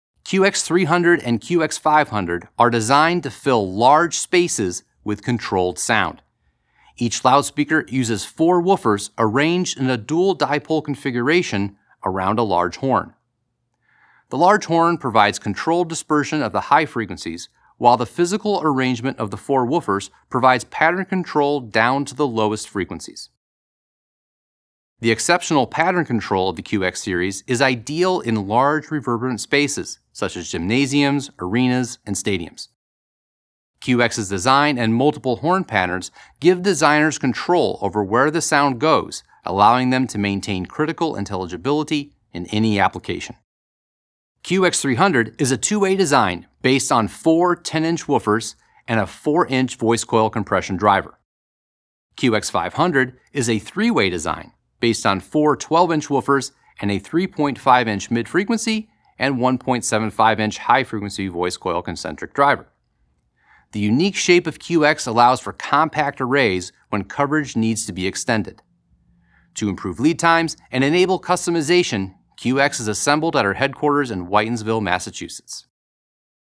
QX Series
The QX series is optimized for installed system applications that require precise directivity in a powerful, low-profile form. ‘Line array’ control and output, without the line array.